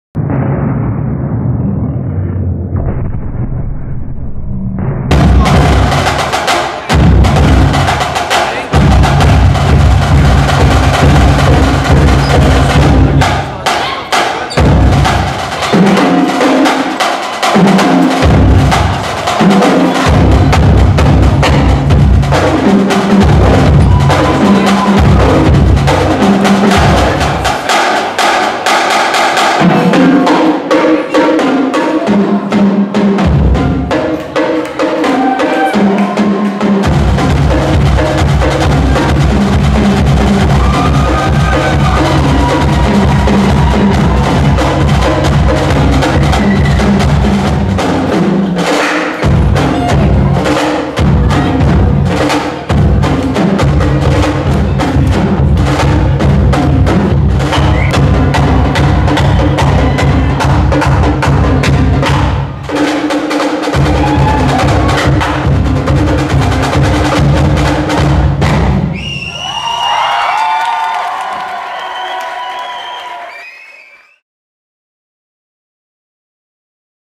Sixpackk - Fanfaronade 2013
Hier endlich das lang erwartete Video der Fanfaronade mit dem Auftritt der Band SIXPACKK